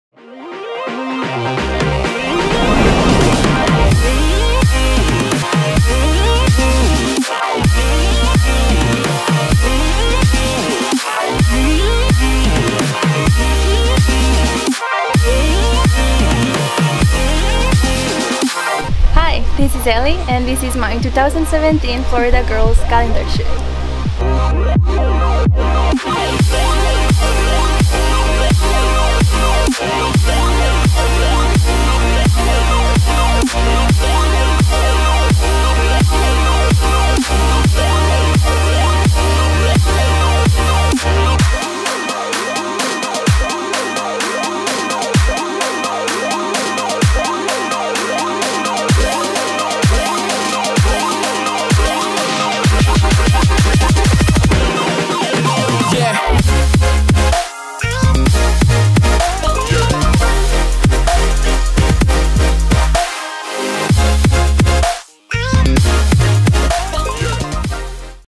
Florida Girls Calendar Photoshoot in 4k! | Ellie Di Virgilio
Behind the scenes with model -  Ellie Di Virgilio

In collaboration with Dion Photography. check out his work on instagram.

this was filmed with the Canon 1DX Mark 2 in 4k and the dji phantom 3 standard drone. For stabilization I used the Devin Graham signature series glide cam.

editing was done using Final Cut Pro X by Trevor McDonald.

Facebook.com/reeltimefilms
Beme- ReelTimeFilms